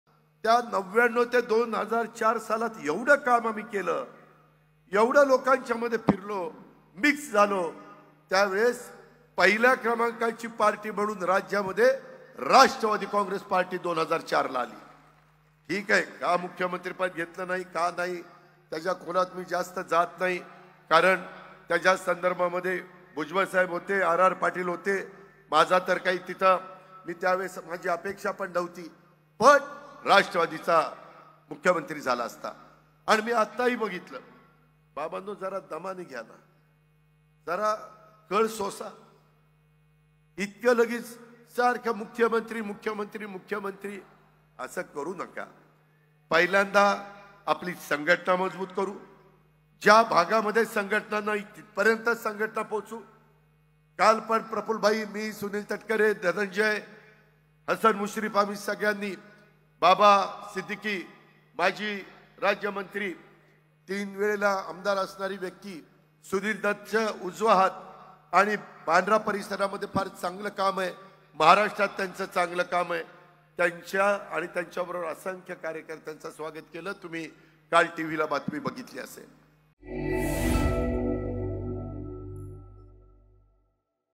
जरा कळ सोसा, सारखं मुख्यमंत्री मुख्यमंत्री करू नका - अजित पवार
पुण्यातील राष्ट्रवादी काँग्रेस अजित पवार गटाच्या युवक मिशन मेळाव्यात कृषीमंत्री धनंजय मुंडे यांनी अजित पवार मुख्यमंत्री होण्याबाबत भाष्य केलं. त्यांच्या या भाष्यानंतर अजित पवारांनी आपल्या भाषणावेळी धनंजय मुंडे यांचे कान टोचले. 'जरा कळ सोसा, सारखं मुख्यमंत्री मुख्यमंत्री करू नका', असे अजित पवार म्हणाले.